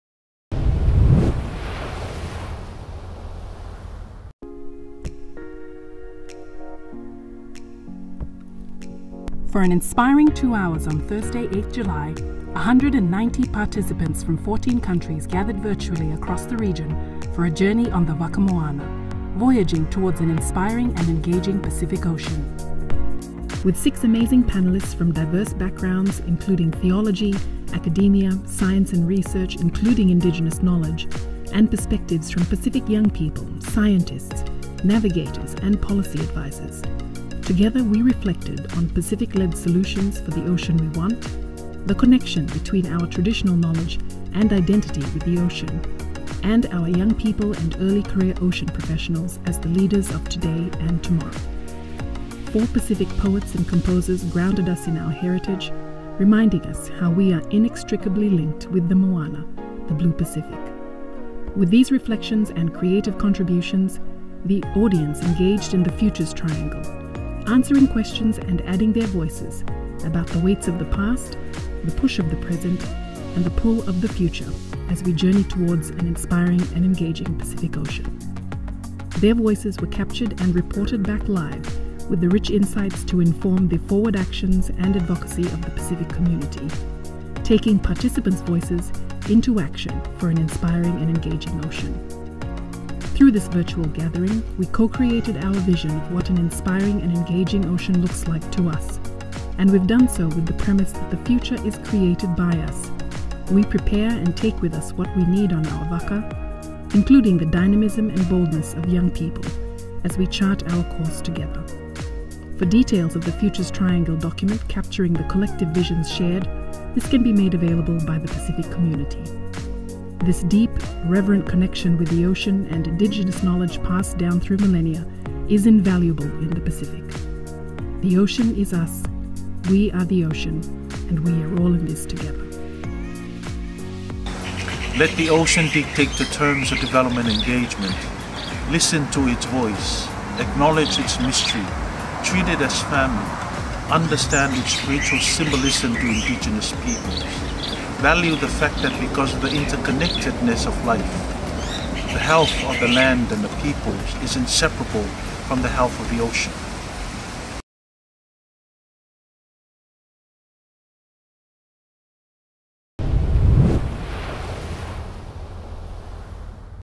Vaka Moana: A journey to an inspiring and engaging Pacific Ocean
For an inspiring 2 hours on Thursday 8 July, 140 participants from 14 countries gathered virtually across the region for a journey on the Vaka Moana...voyaging towards an inspiring and engaging Pacific Ocean.

With 6 amazing panellists from diverse backgrounds in the region including theology, academia, science and research, including indigenous knowledge, and perspectives from Pacific young people, scientists, navigators and policy advisors - together we reflected on Pacific-led solutions for the ocean we want, the connection between our traditional knowledge and identity with the ocean, and our young people and early career ocean professionals as the leaders of today and tomorrow.